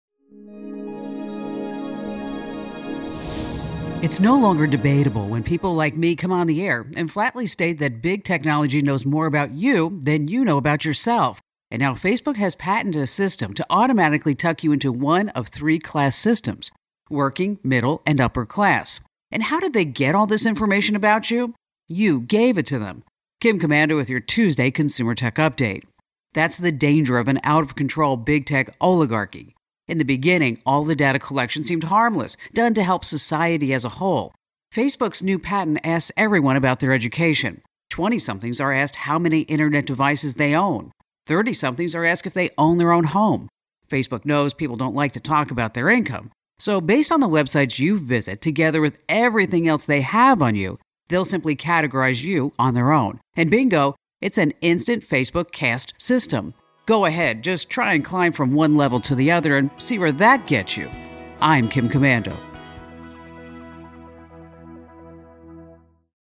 Big tech knows more about you than you think
The tricky ways big tech profiles you